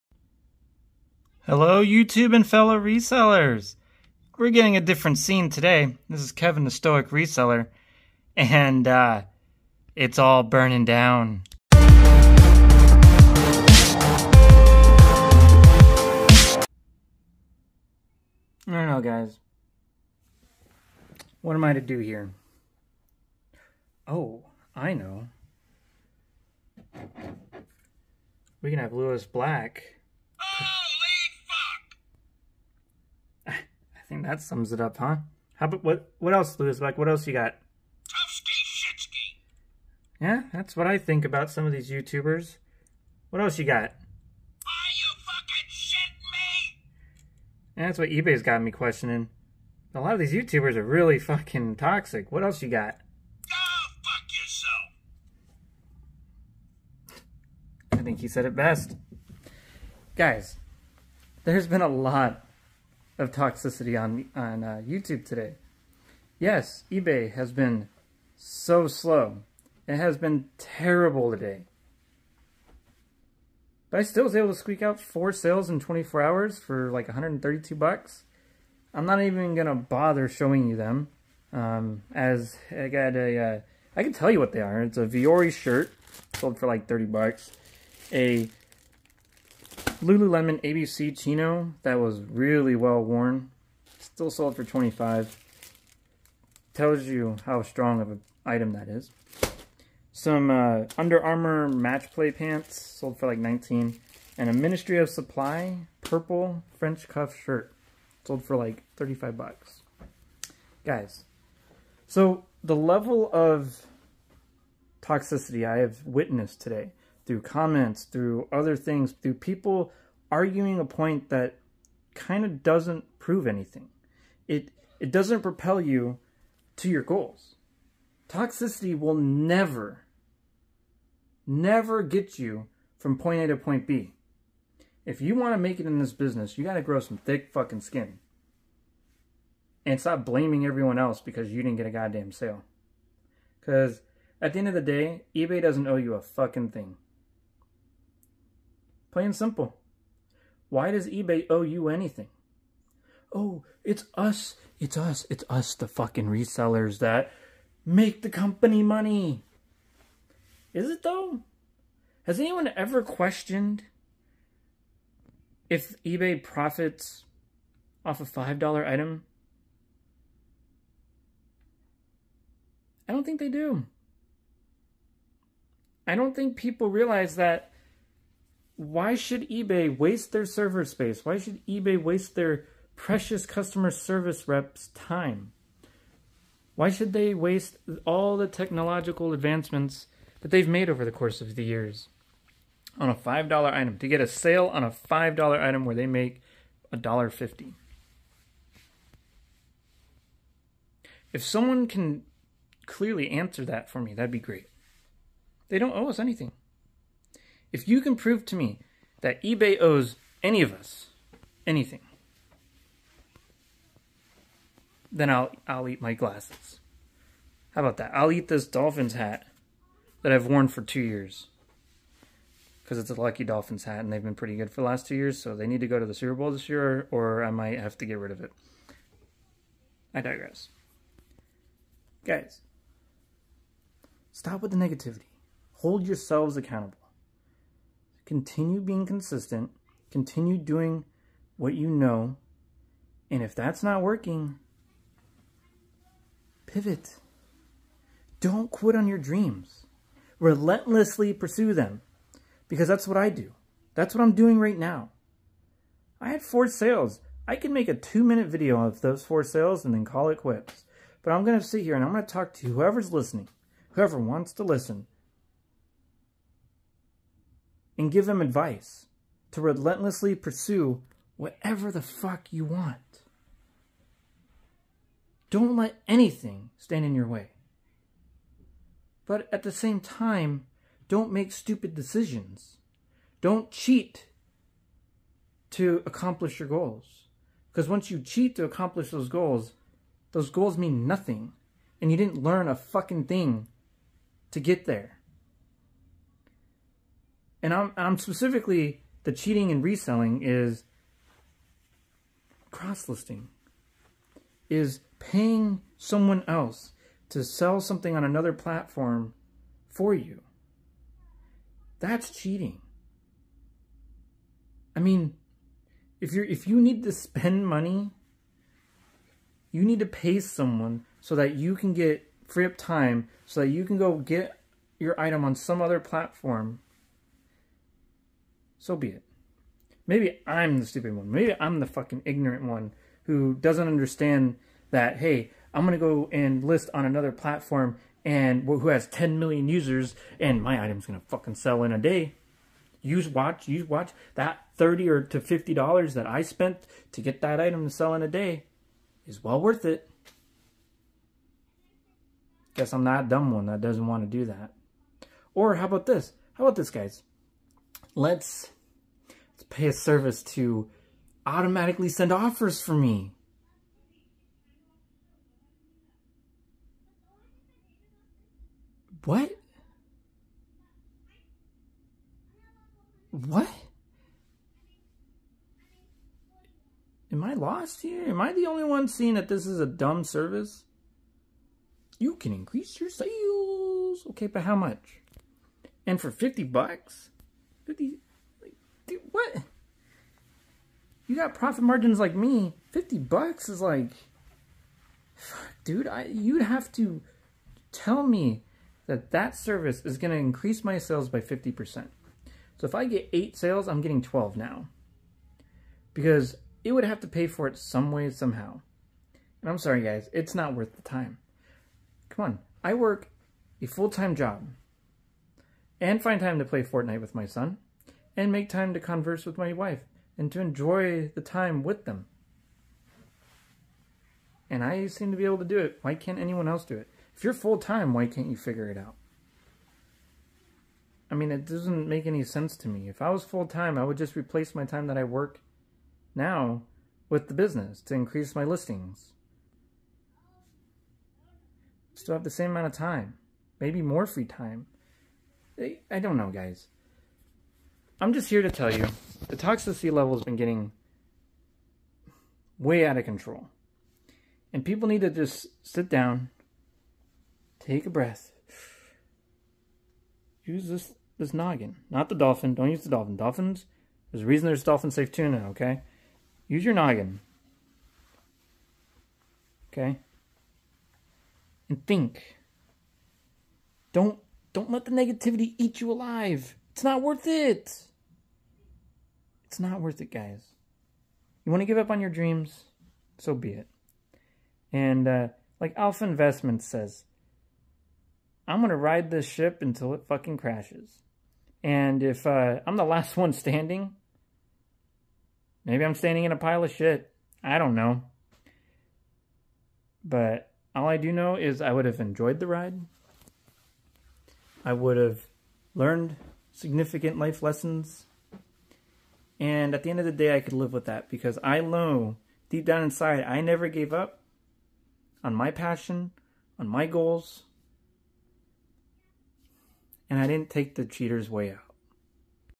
This Has To Be The End Right? Toxic Rant.... EBAY What Sold In Last 24 Hours. 8-2-23
In this episode we talk to Lewis Black about the toxic nature of Youtubers and EBAY. Should you be worried? Probably not but the toxicity being driven in our reselling community is at an all time level. EBAY isn't the problem, YOU are!